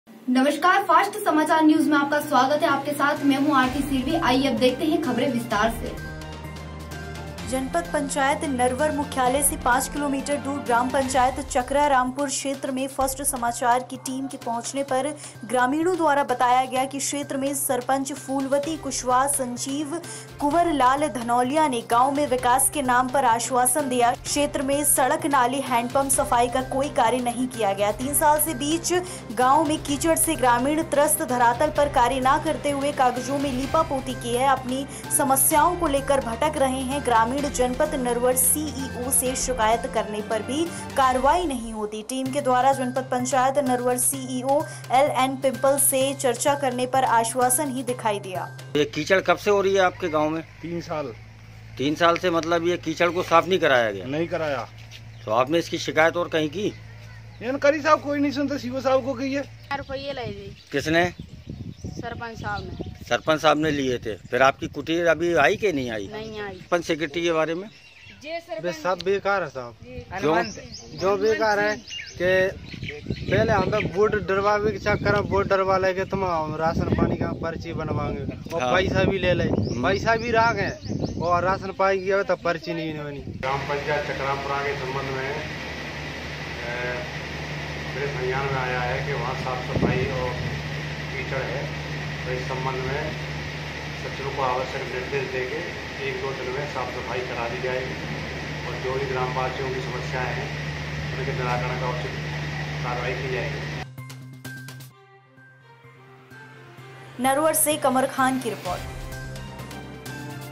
नरवर - रामपुर सरपंच सचिव द्वारा क्षेत्र में नहीं किए गए विकास कार्य आदेशों का उल्लंघन
जनपद पंचायत नरवर मुख्यालय से 5 किलोमीटर दूर ग्राम पंचायत चकरामपुर क्षेत्र में फ़ास्ट  समाचार की टीम के पहुंचने पर ग्रामीणों द्वारा बताया गया क्षेत्र में सरपंच फूलवती कुशवाह सचिव कुंवर लाल धनौलिया ने गांव में विकास के नाम पर आश्वासन दिया क्षेत्र में सड़क नाली हेडपंप सफाई का कोई कार्य नहीं किया गया 3 साल से बीच गांव में कीचड़ से ग्रामीण त्रस्त धरातल पर कार्य न करते हुए कागजों में लीपा पोती की गई है अपनी समस्याओं को लेकर भटक रहे ग्रामीण जनपद नरवर सीईओ से शिकायत करने पर भी कार्यवाही नहीं होती टीम के द्वारा जनपद पंचायत नरवर सीईओ एलएन पिंपल से चर्चा करने पर आश्वासन ही दिखाई दिय